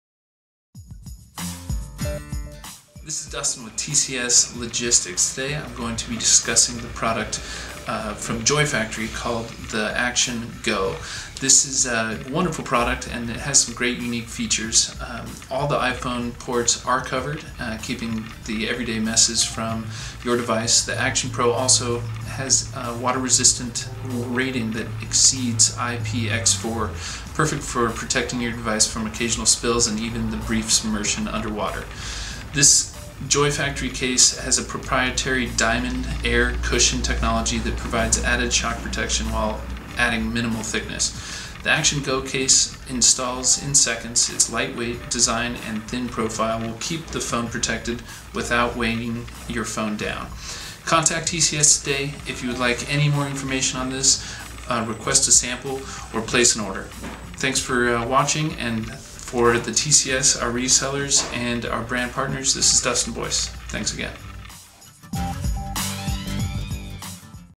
TCS Logistics presents the aXtion Go for iPhone 5 from Joy Factory
The aXtion Go for iPhone 5 is the most unique case to be released in years. Bridging the thin, stylish contour of a fashion case and the full-orbed protection of a rugged case, the aXtion Go is the one case you'll need for everyday protection wherever you may go.